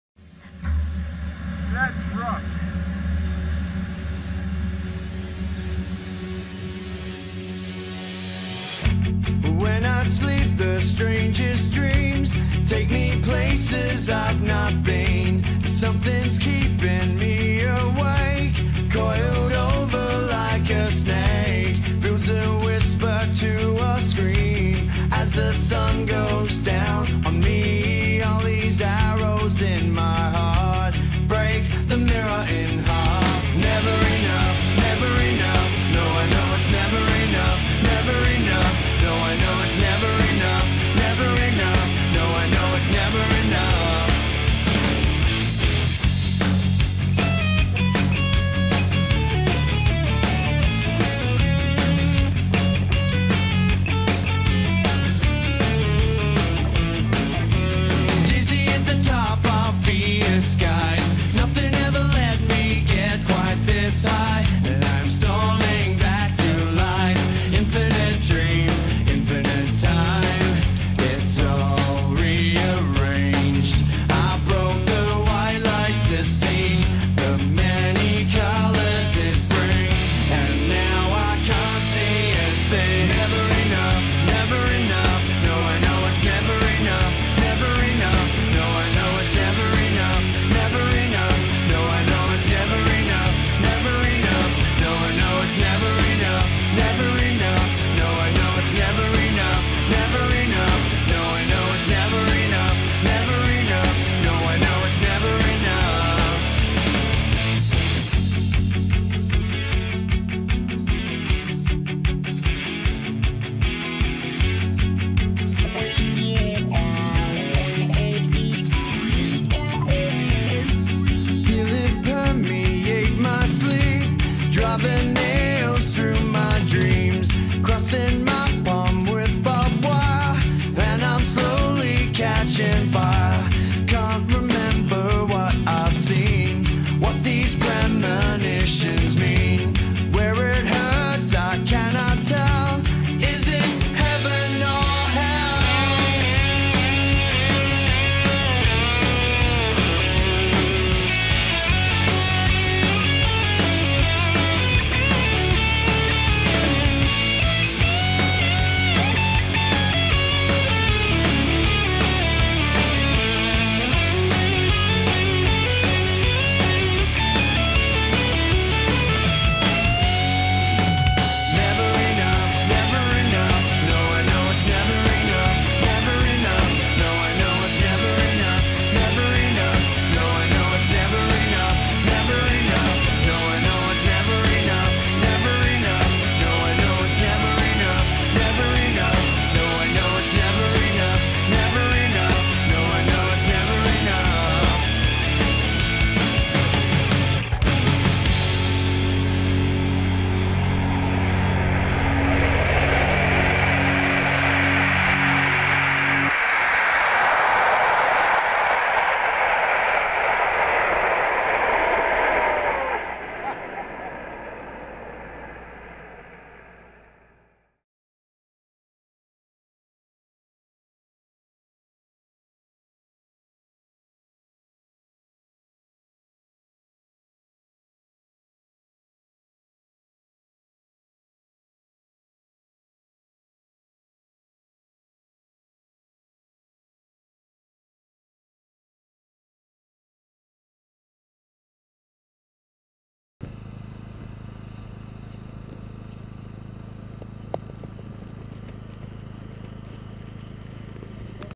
Greenland 2012 - Field Expedition NEEM
Some impressions from the 2012 field campaign at Greenland NEEM. Polar 6, polar aircraft footage.